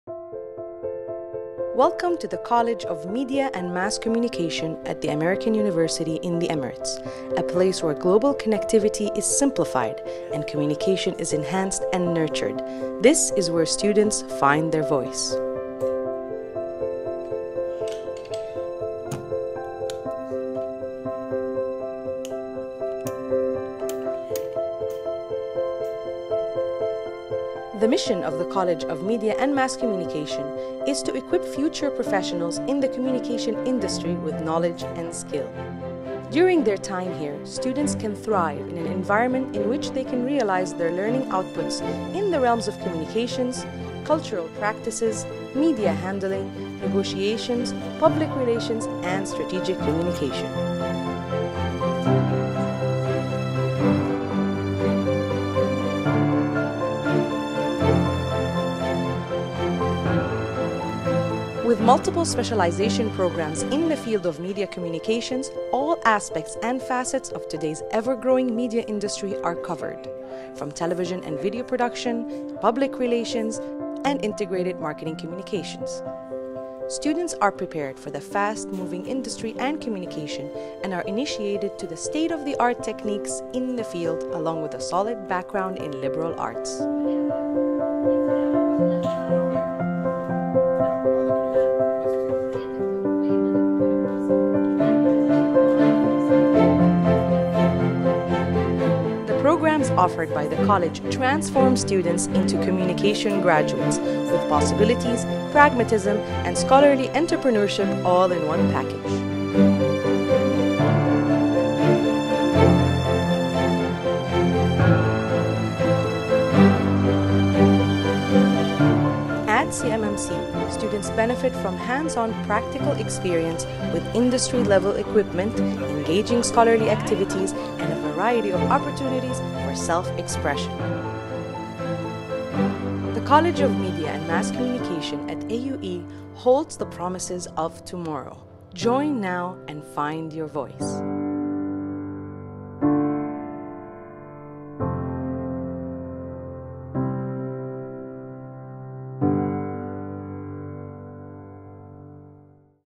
Study BA Radio & TV at the College of Media and Mass Communication.
Do You Want to be a Top TV & Film Director or Produce Your Own Radio Show?
Study BA Radio & TV at the College of Media and Mass Communication and get the skills to start your career.